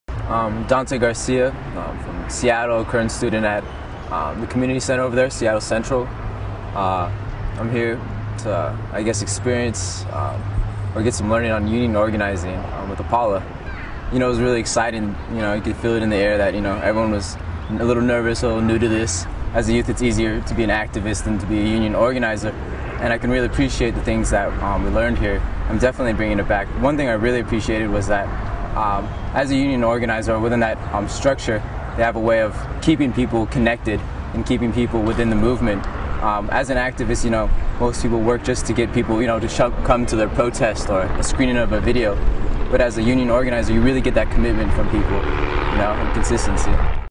APALA OI Testimonial: Dante Garcia
Dante Garcia, a Seattle native, relates how his views on activism and organizing have changed because of his experience at the 2008 APALA Organizing Institute.